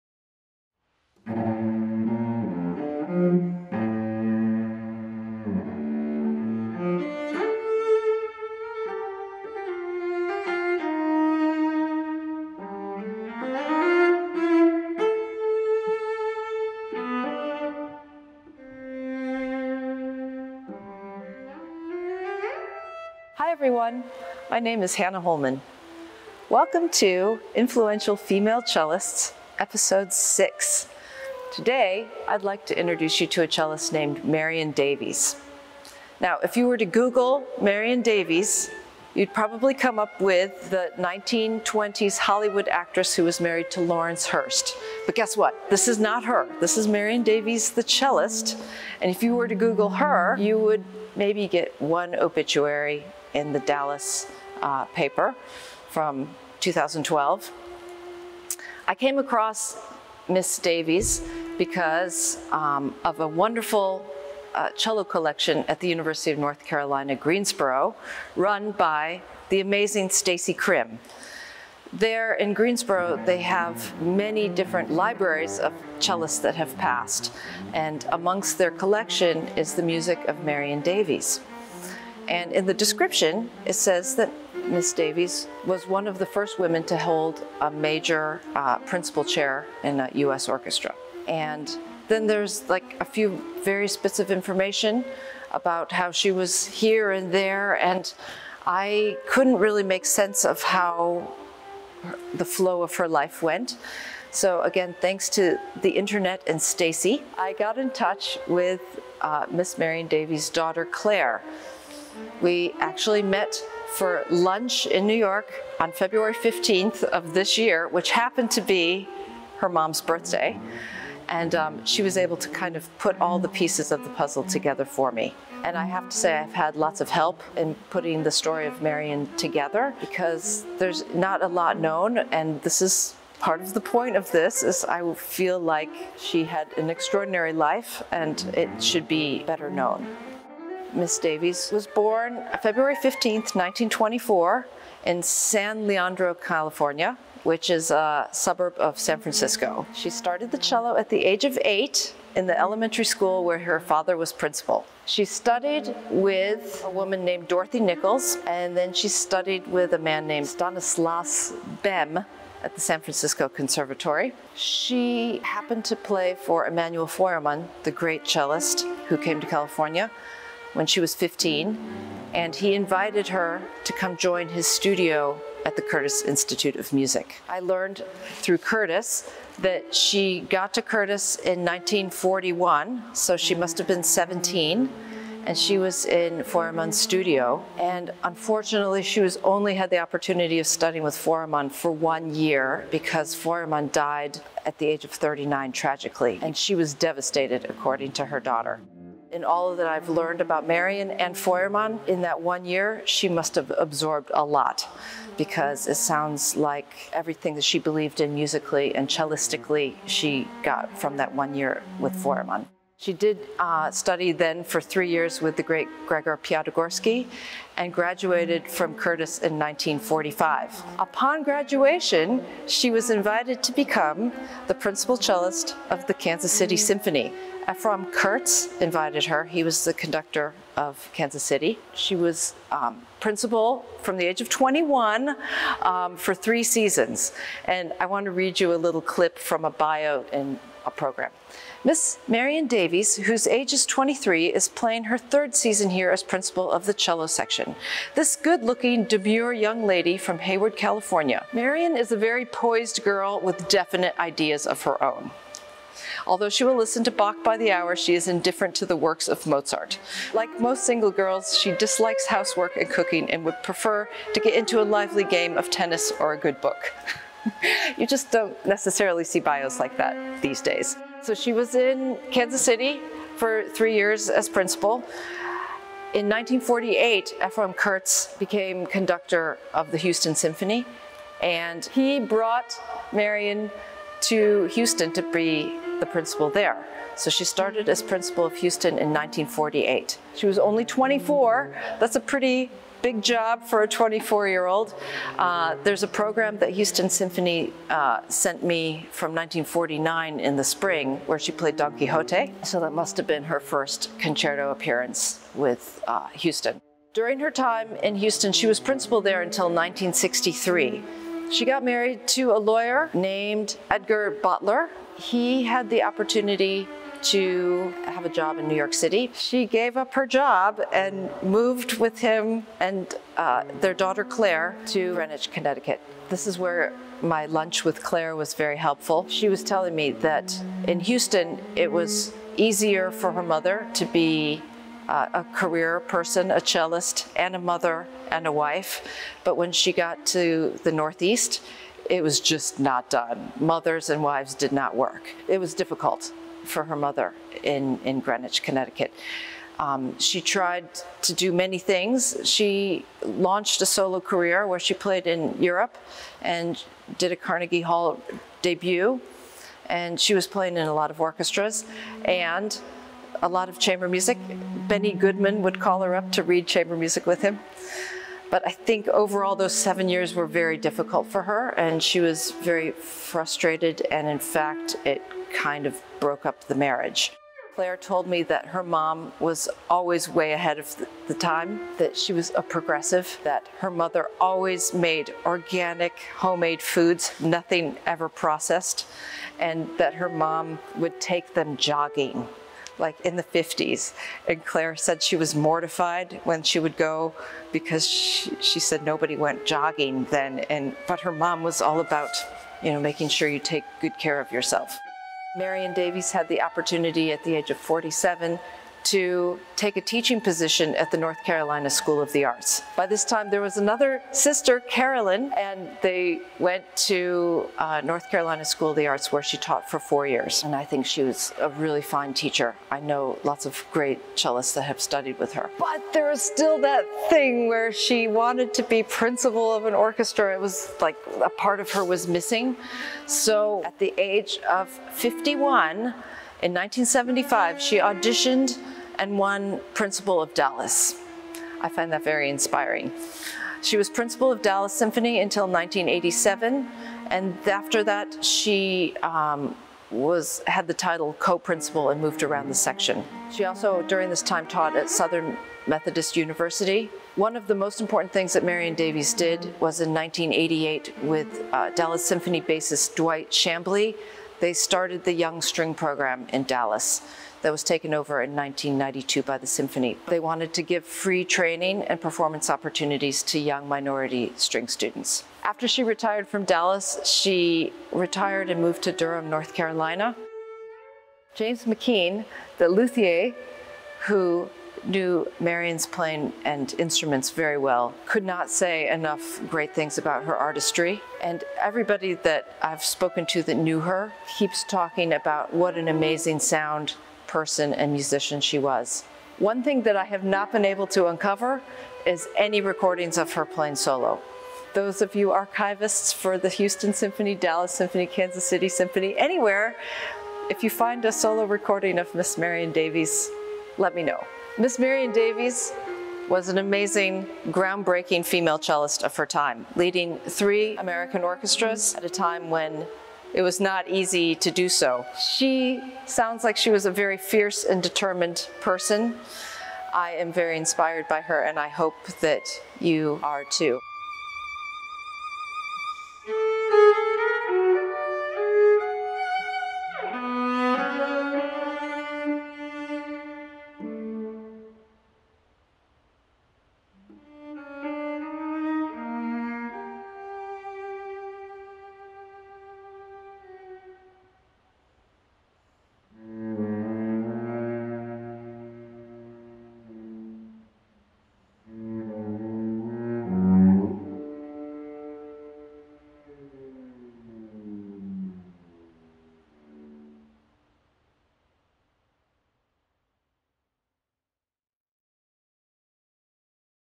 Influential Female Cellists with Hannah Holman, Episode 6 ~ Marion Davies
the sixth in a series highlighting the lives of women cellists of the past, some more known than others.

Marion was a student of Feuermann and Piatigorsky, principal cellist of the Kansas City Symphony, the Houston Symphony and the Dallas Symphony!  what an achievement!!